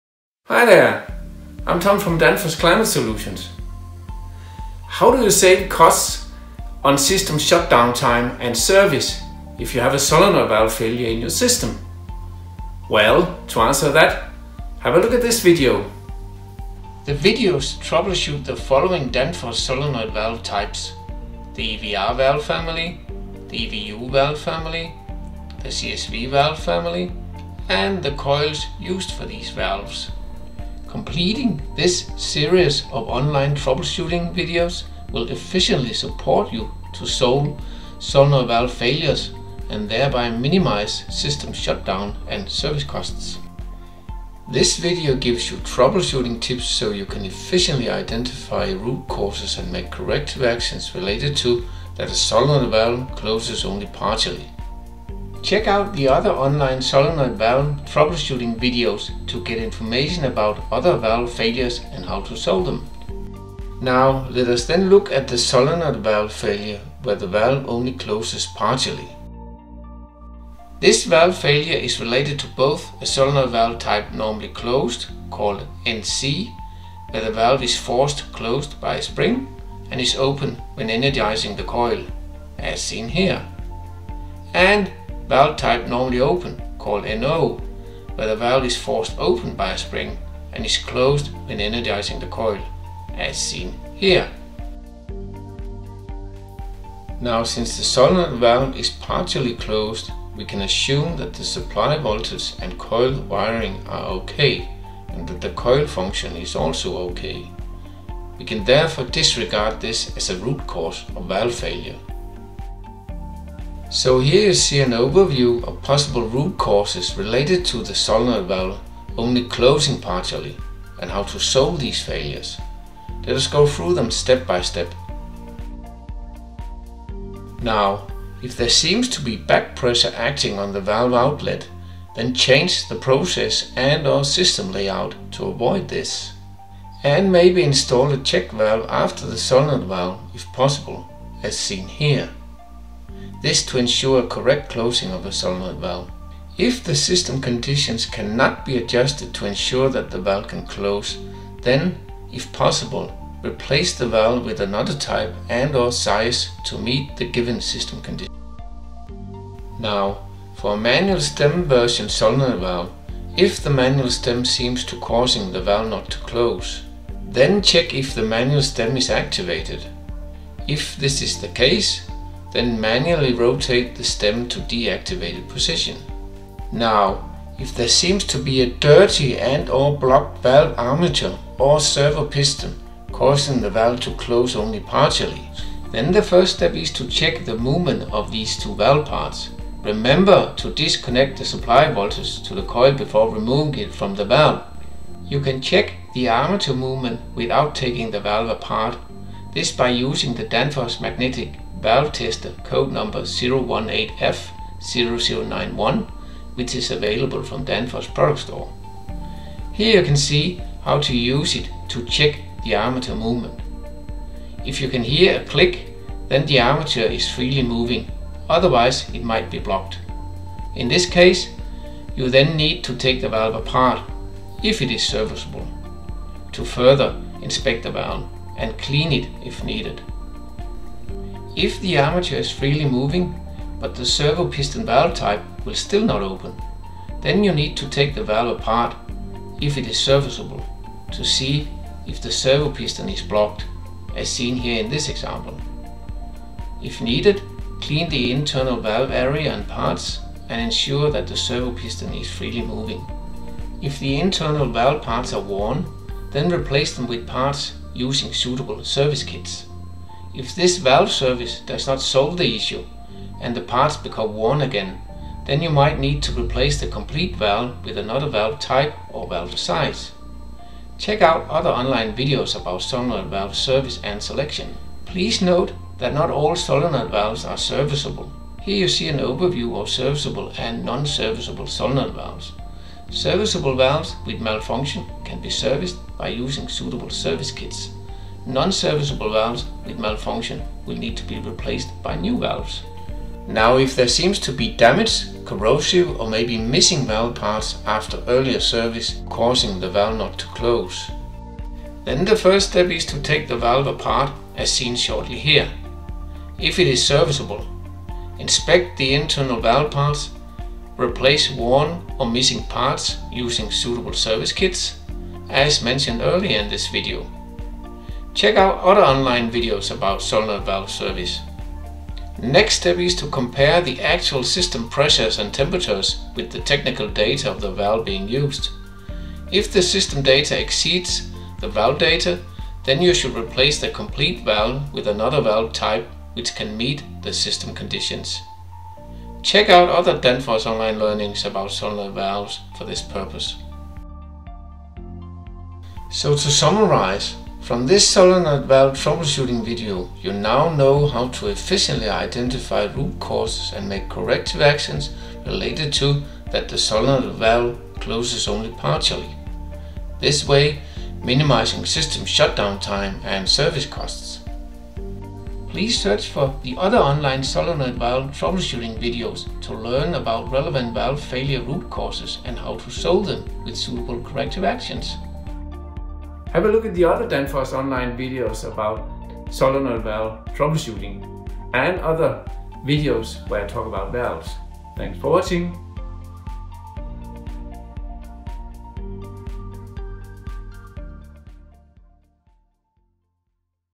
How to troubleshoot a solenoid valve that is only partially closed | HVAC&R Distillery
This video gives you troubleshooting tips related to solenoid valves that are only partially closed.

Tom Jensen provides valuable insights in how to troubleshoot solenoid valves that are only partially closed—highlighting possible root causes and solutions for your benefit. Tom talks about back pressure, manual stem, operation, and much more. He walks you through the corrective actions step-by-step—checking valve parts and doing valve service being some of them.

Chapters:
00:00 Introduction
00:20 Danfoss solenoid valve types
00:52 Valves closes only partially
01:56 Overview of possible root causes
02:07 Too high back pressure
02:41 Manual stem activation
02:57 Dirty/blocked armature or servo piston
04:54 Damaged, corrosive, or missing parts
05.:45 Lessons learned